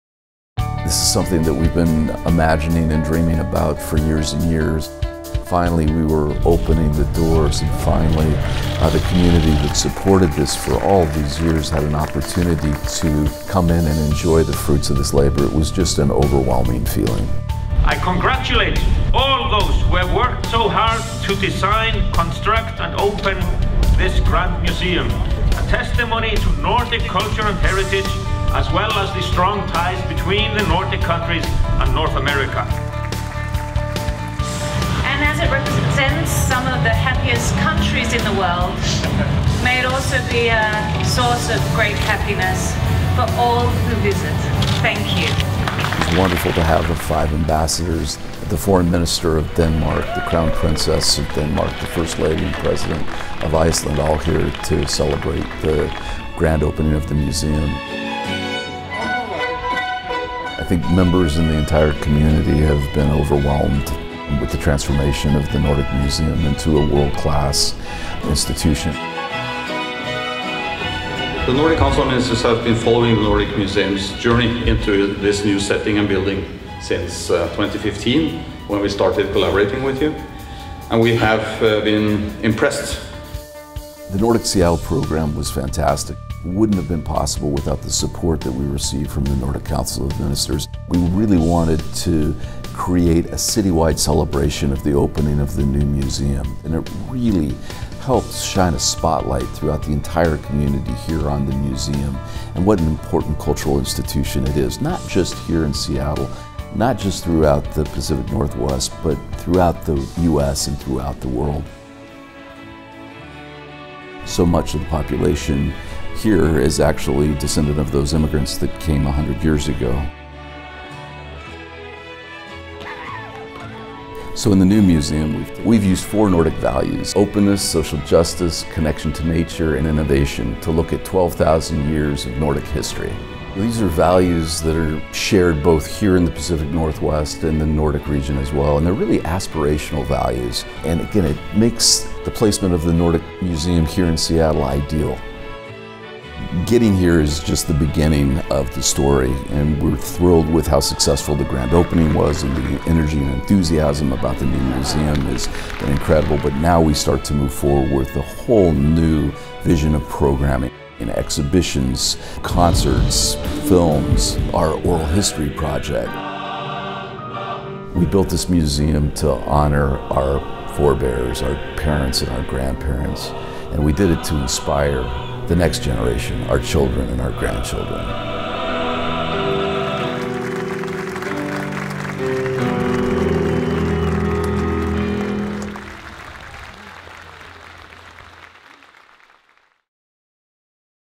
Grand Opening Recap
Executive Director and CEO Eric Nelson shares the success of the Museum's grand opening, and how our mission and values guide our institution.